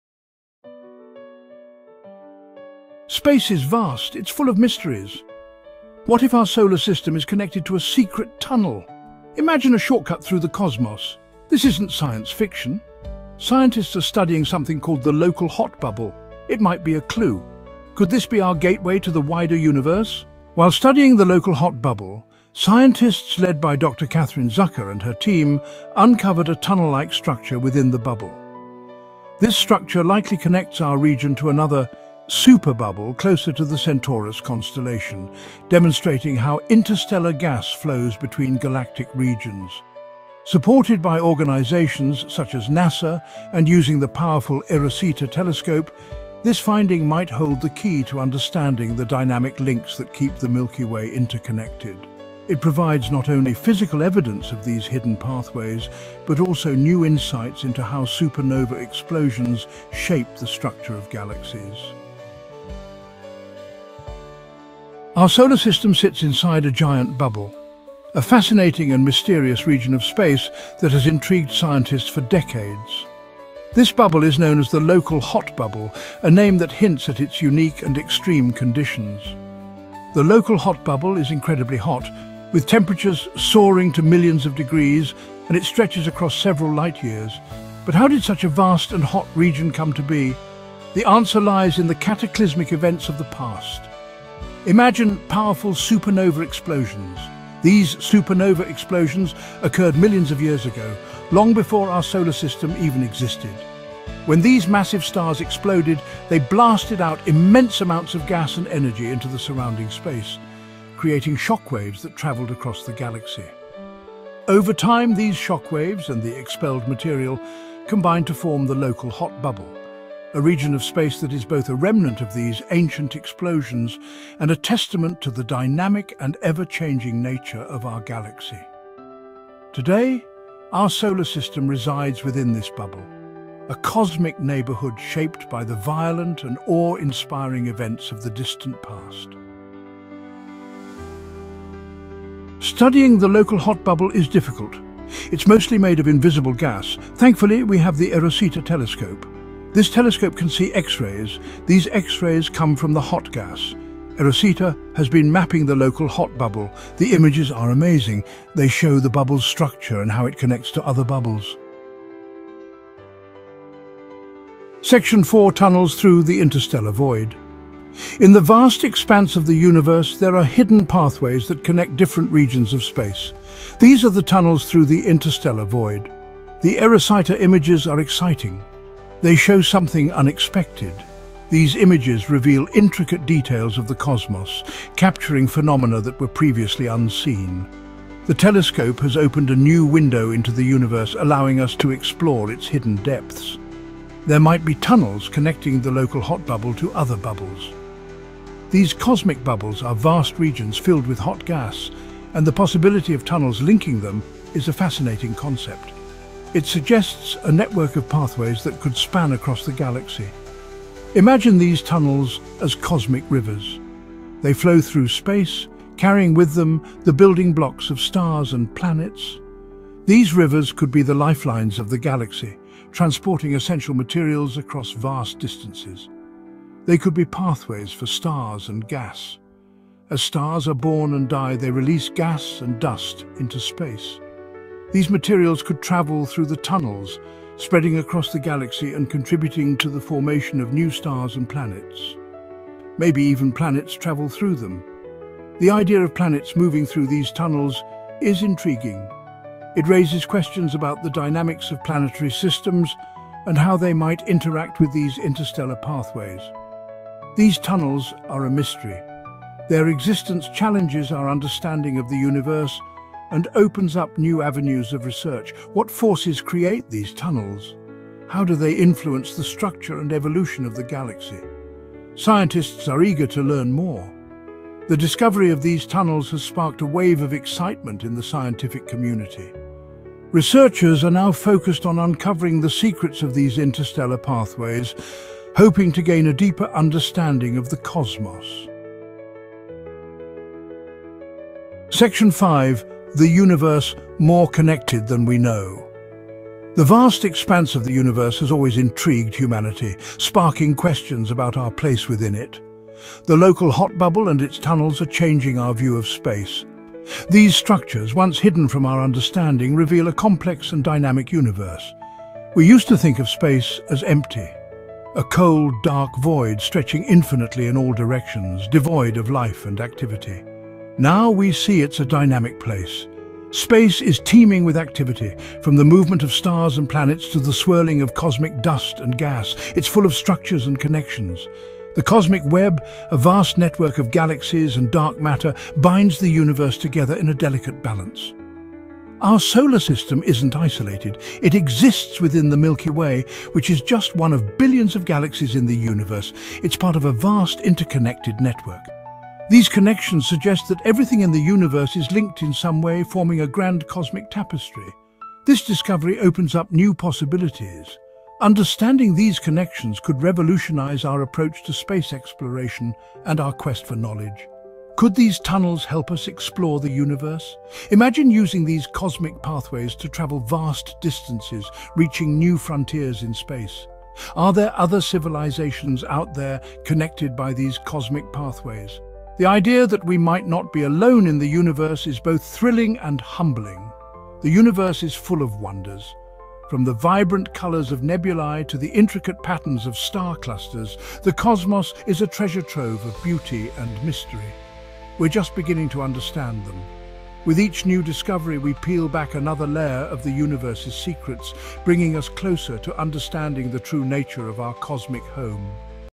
"Mysterious Interstellar Tunnel Discovered by Erosita Telescope"
00:00:00
A Cosmic Gateway?

00:00:20
New Discovery: The Interstellar Tunnel

00:01:02
Explosions That Shaped Our Cosmic Neighborhood

00:02:19
Mapping the Invisible with eROSITA

00:02:41
Tunnels Through the Interstellar Void

00:04:48
More Connected Than We Know

Scientists have made an astounding breakthrough: the eROSITA telescope has uncovered a mysterious interstellar tunnel hidden within the Local Hot Bubble, a region near our Solar System. This mysterious interstellar tunnel, discovered with the help of the advanced eROSITA telescope, could connect us to previously unexplored parts of the Milky Way.

The mysterious interstellar tunnel resides in a low-density, X-ray-emitting area that scientists call the Local Hot Bubble, which was formed by supernova explosions millions of years ago. With the eROSITA telescope, researchers detected this tunnel-like structure, providing invaluable data about interstellar gas, galactic formation, and the dynamic nature of our galaxy. This discovery is rewriting what we know about the Milky Way.

The eROSITA telescope played a pivotal role in identifying this mysterious interstellar tunnel through its advanced X-ray detection capabilities. This tunnel could represent a new pathway between galactic regions, suggesting an intricate connection between different parts of the Milky Way. Such findings hint at larger intergalactic networks that challenge everything we thought about space exploration.

For space exploration and astronomical research, the eROSITA telescope’s discovery of the mysterious interstellar tunnel is a momentous achievement. It unveils how supernova remnants and high-energy activity contribute to the formation of tunnels and cavities in the galaxy. Scientists are now hopeful this mysterious interstellar tunnel, mapped by the eROSITA telescope, will shed light on how energy flows across the galaxy and the potential for future space travel through similar structures.

The mysterious interstellar tunnel discovered by the eROSITA telescope demonstrates humanity's drive to explore the cosmos. As we unravel these mysteries, we move closer to uncovering the secrets of the Milky Way and our universe's vast network.

Extensive study